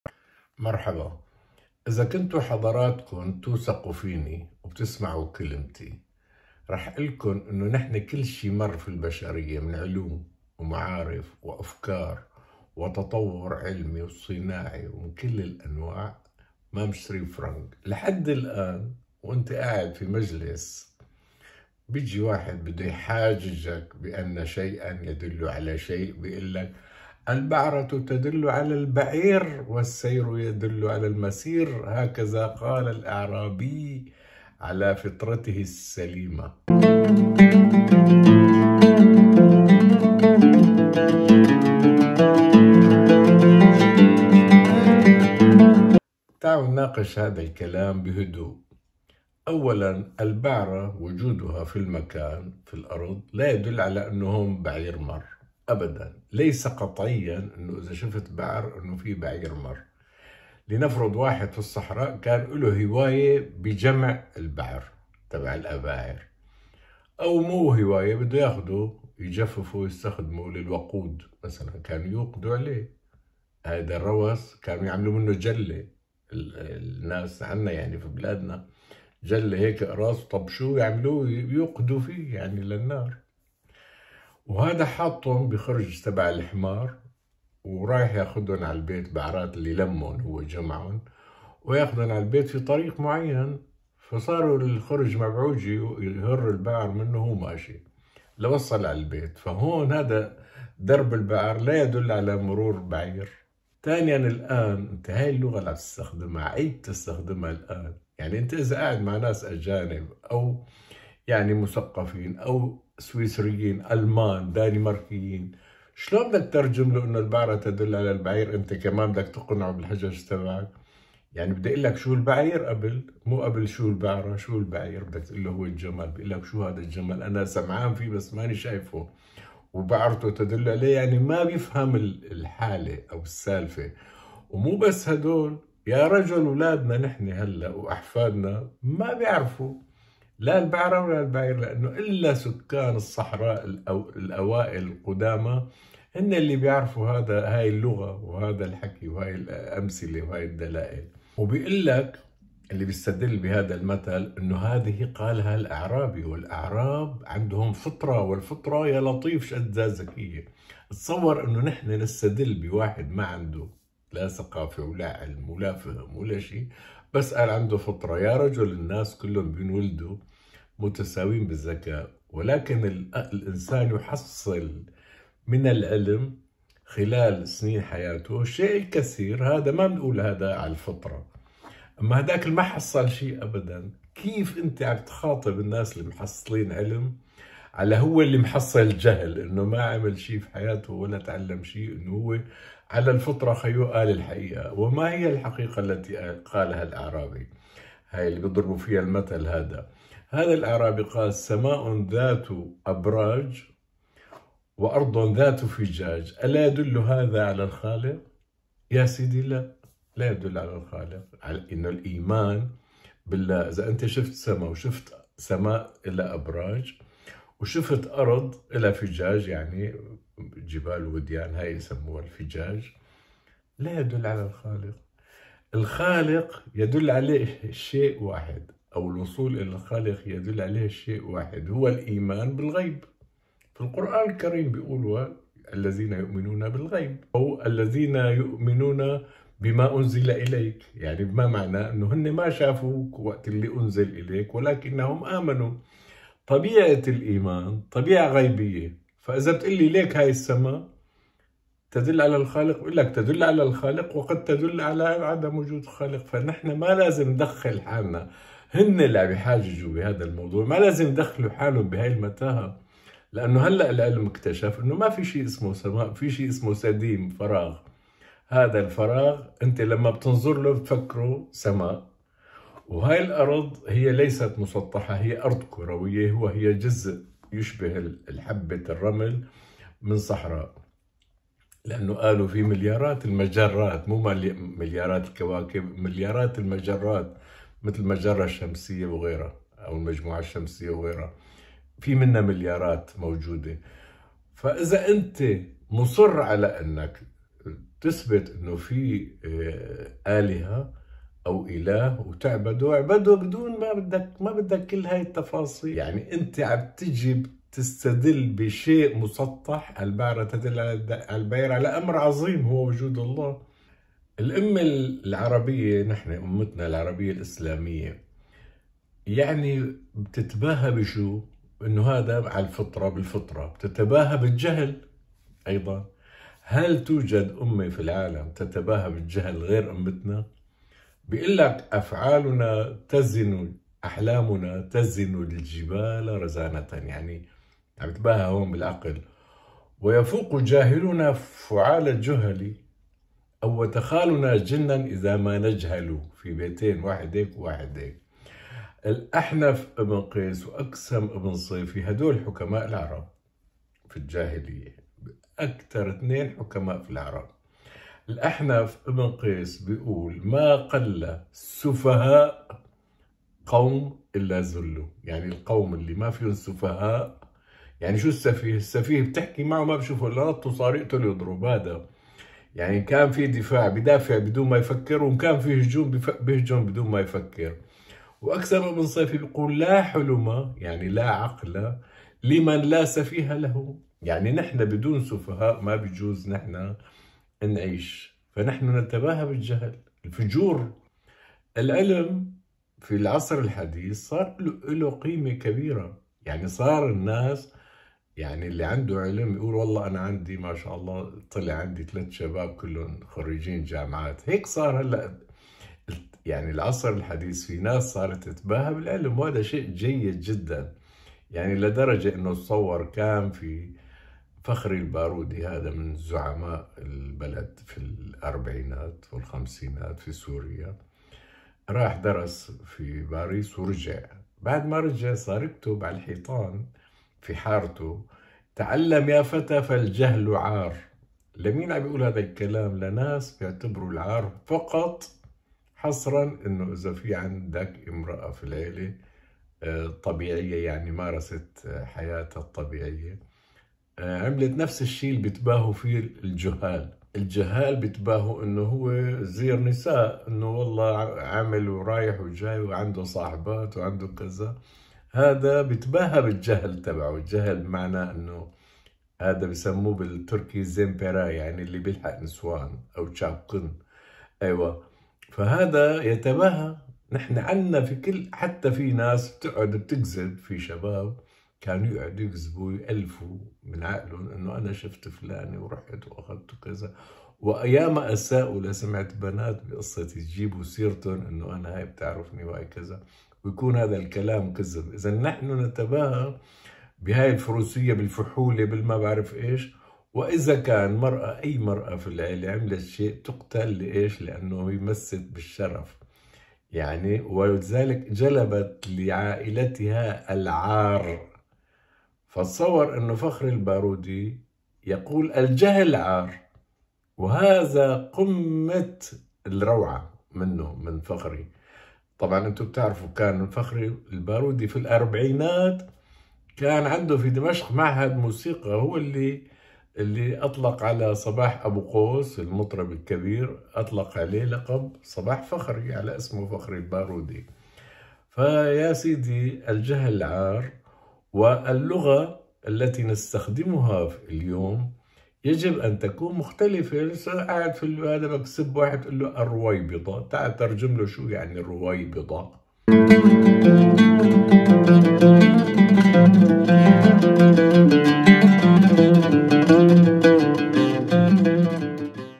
الأعرابي الرائع- وحكاية البعرة التي تدل على البعير
قد تجد بعراً في الصحراء كان قد وضعه رجل ما في الخرج على ظهر حمار، والخرج مخروق، ما يعني أن البعرة تدل على الجحش.